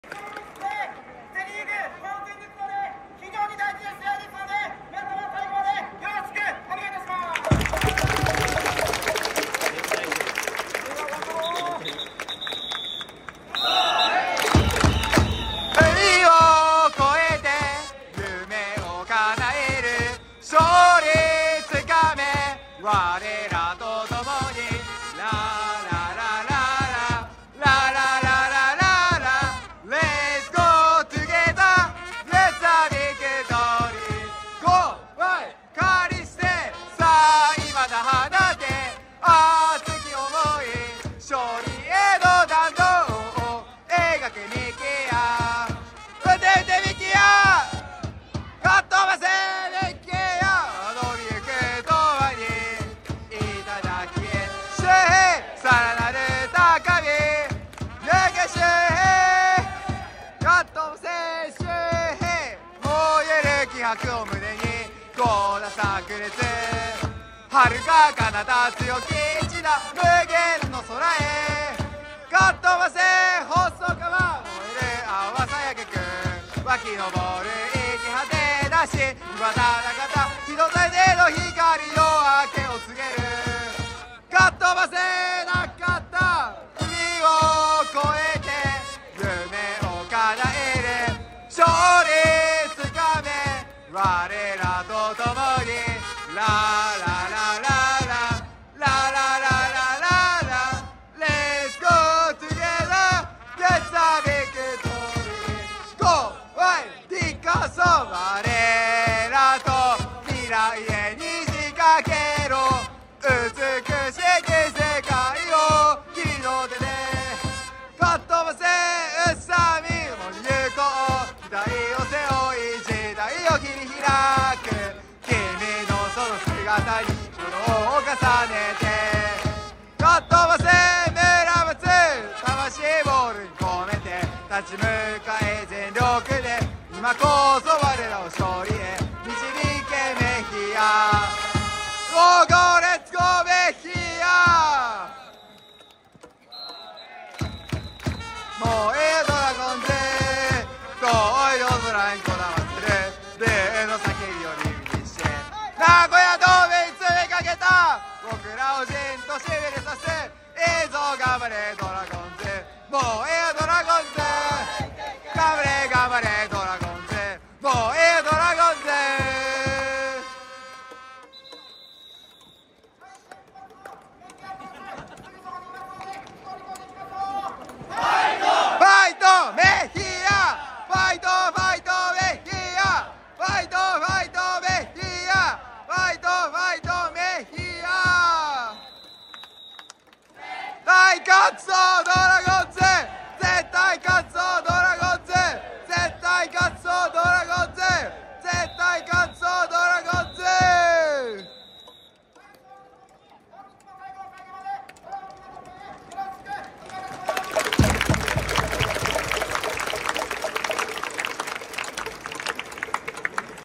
6/22 中日 1-9
6/22 中日vs広島(バンテリンドームナゴヤ)

一(中)カリステ
二(二)田中
三(三)高橋周
四(右)細川
五(一)中田
六(左)ディカーソン
七(補)宇佐見
八(遊)村松
九(投)メヒア